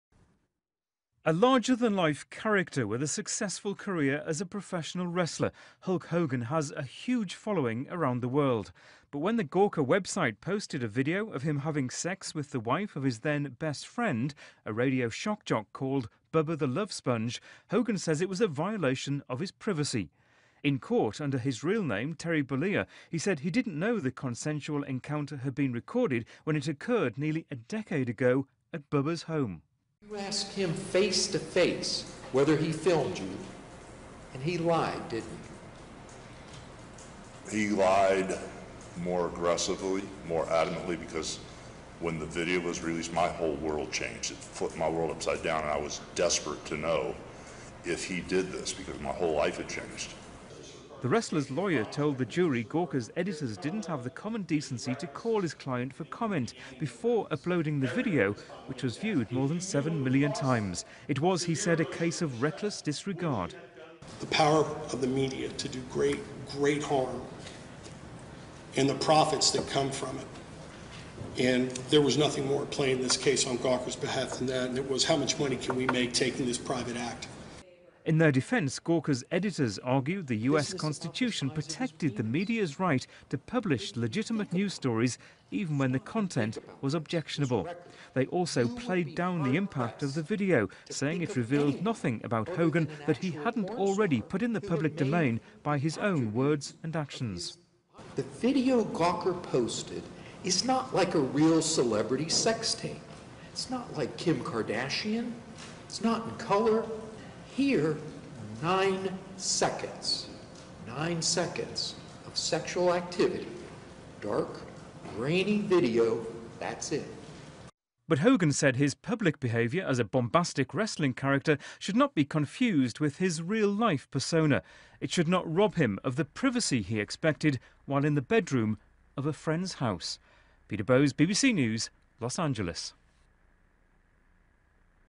Hulk Hogan awarded $115m in Gawker sex tape case
A Florida jury has awarded Hulk Hogan $115m (£79m) after the gossip news website Gawker published a sex tape of the retired professional wrestler.
Mr Hogan's legal team argued the New York-based website violated his privacy and the video was not newsworthy.
The case, which pitted freedom of the press against a celebrity's right to privacy, has been closely watched.
Peter Bowes reports.